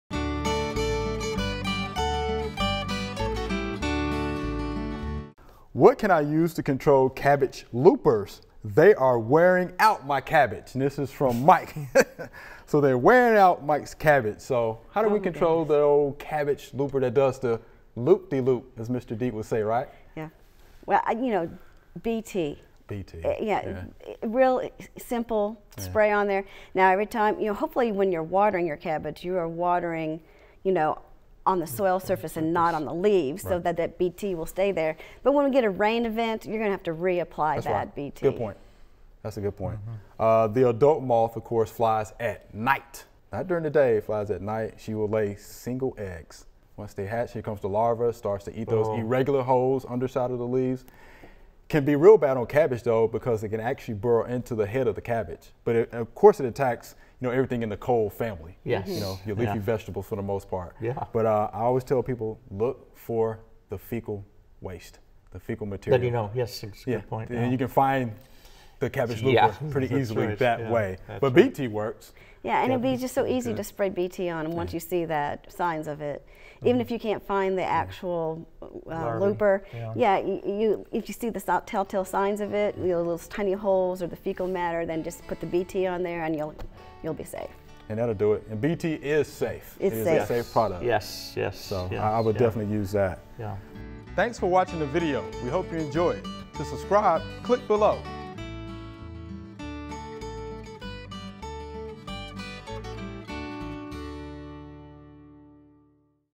Q&A – What can I use to control cabbage loopers?
TSU Extension Agent Joellen Diamond recommends using Bt (Bacillus thuringiensis). UT Extension Agent Chris Cooper gives an overview of the leaf cycle of the cabbage looper and how to find them.